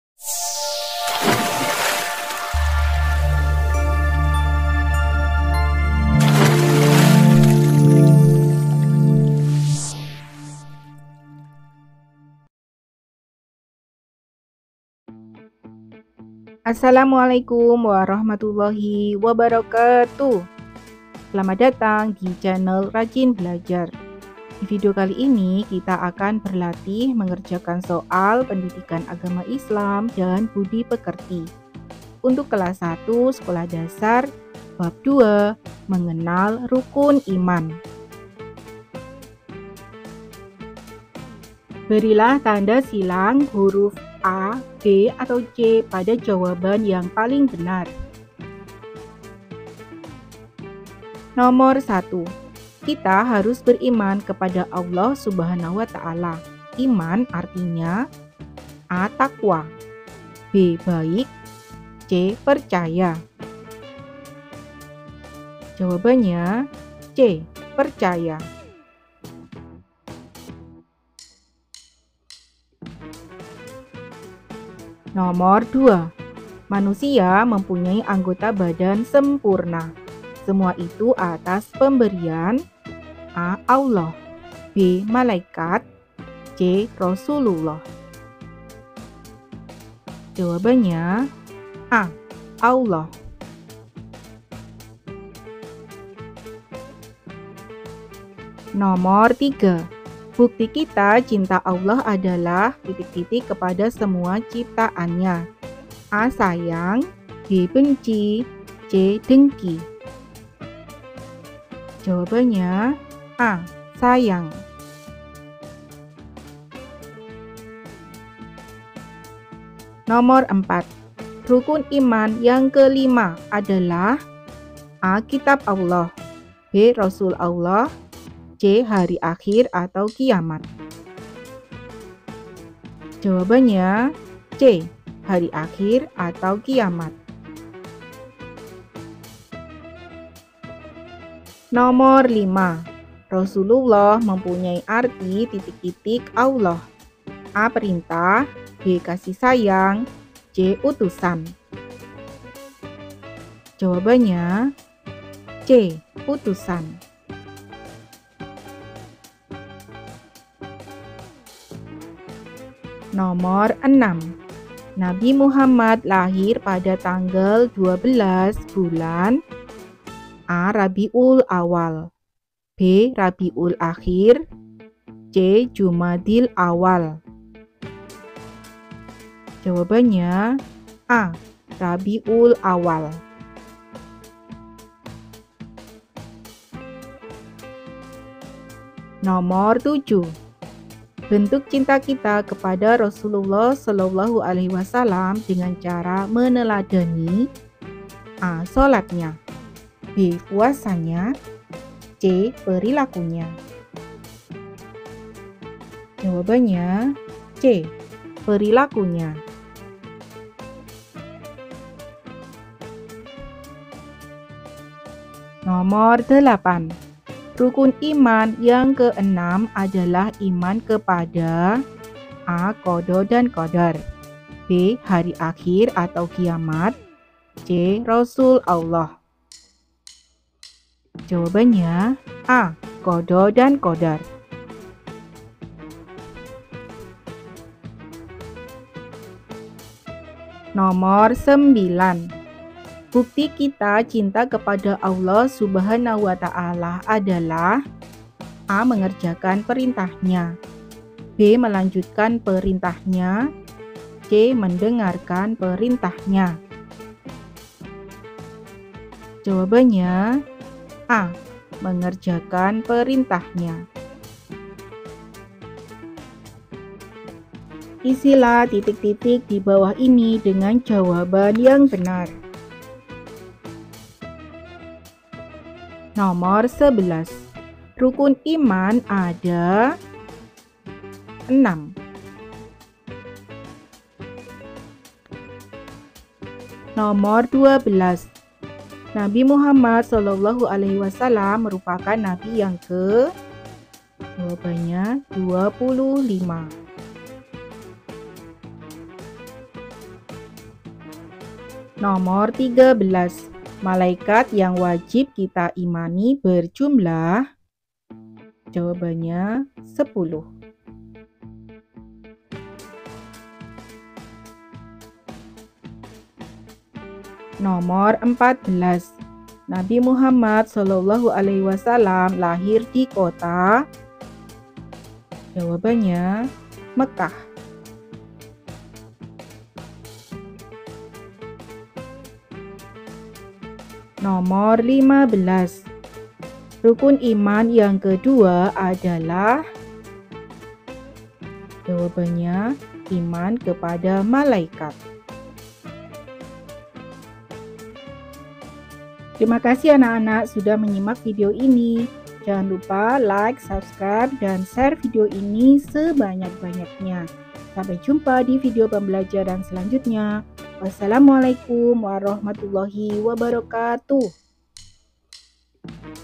Latihan Soal PAI Kelas 1 SD Bab 2 : Mengenal Rukun Iman || Semester 1 - Kurikulum Merdeka
Sumber Soal : Soalpendidikan.net

Semoga bermanfaat ya anak-anak :)
Terima kasih.